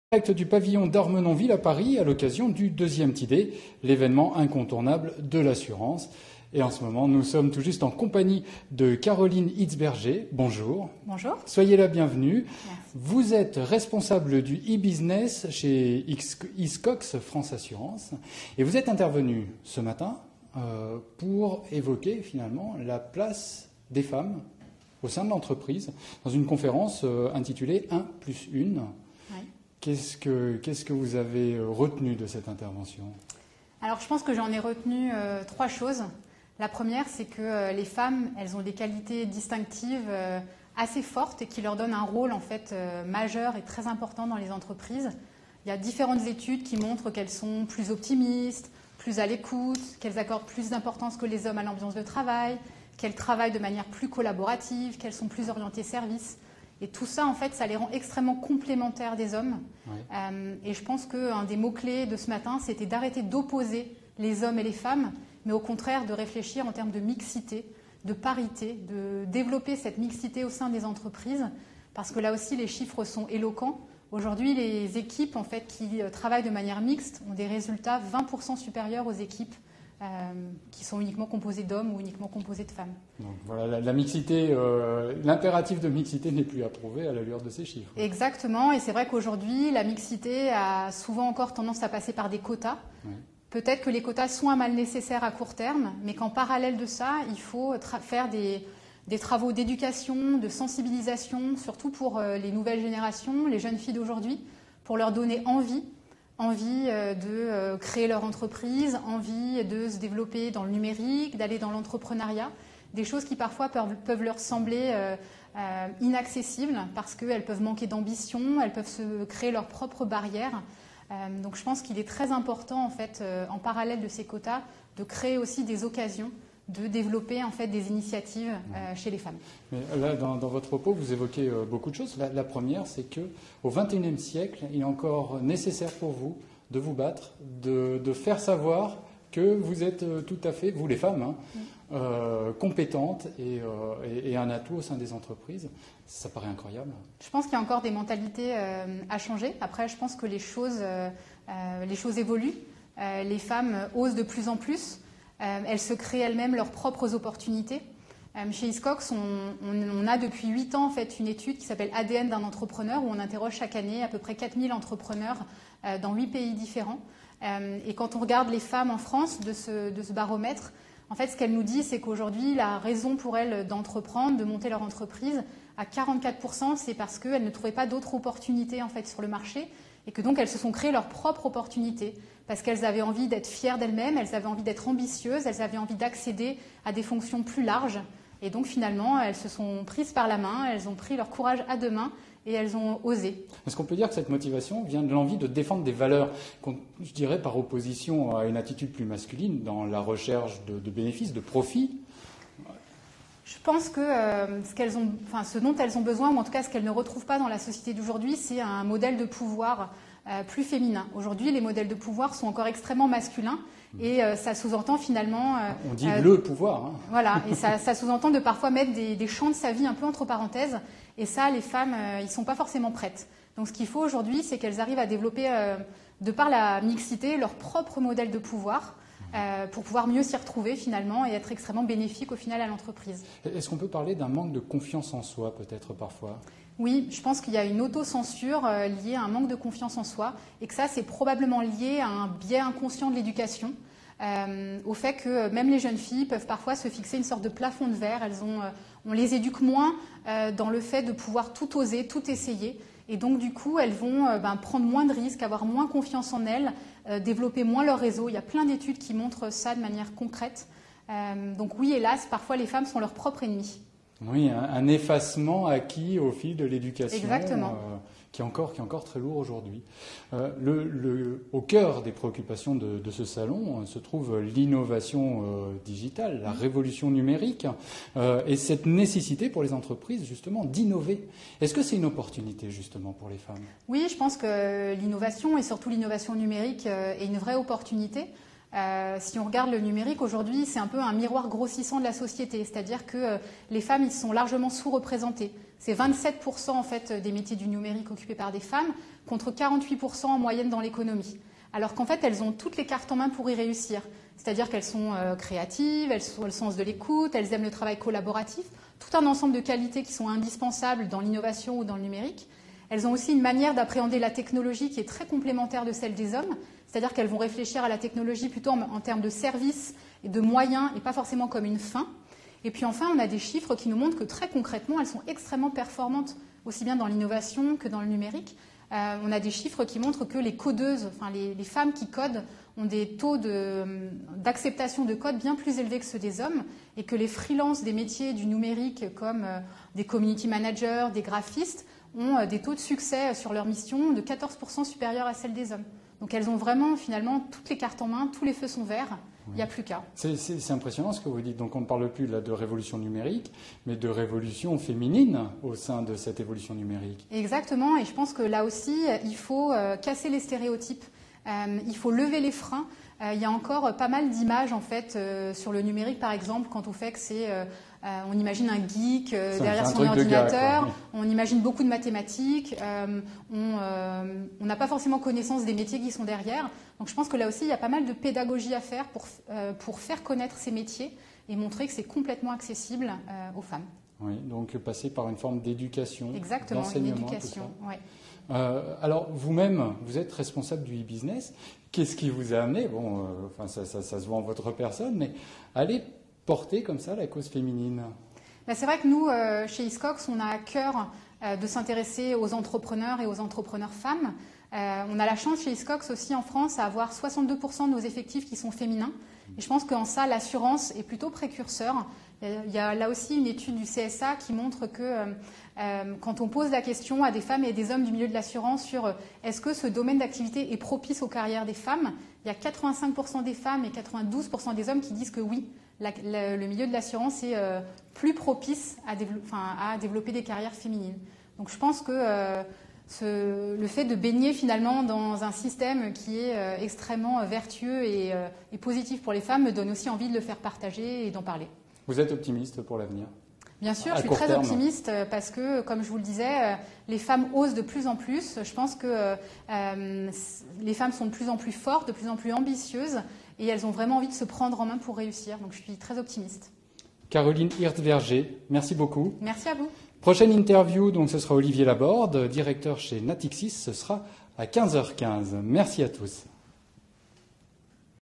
CAROLINE HIRTZBERGER - HISCOX FRANCE ASSURANCES
Enregistrement vidéo du Periscope Live effectué lors du TDAY Insurance du 7 et 8 Juin 2017 organisé par Emmanuelle Lambijou et Jean-Luc Gambey.